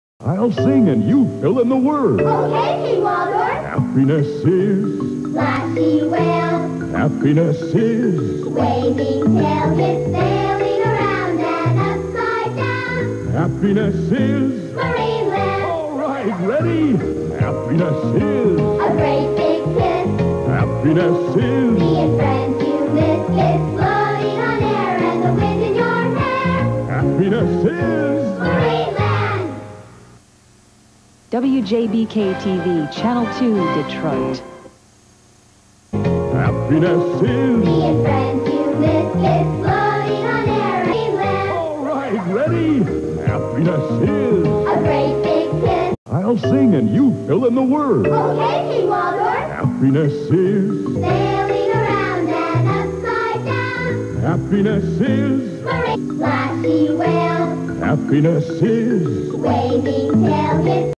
Marineland commercial (1987)
Another one of King Waldorf's Happiness is. commercials.

My name is Ed Saxon. I was a diver for Marineland Of The Pacific from1977 until the Park closed in Feb. 1987. I am presenting a TV commercial for Marineland .

A classic updated version for Marineland Canada. Everyone Love's Marineland

Sky Screamer!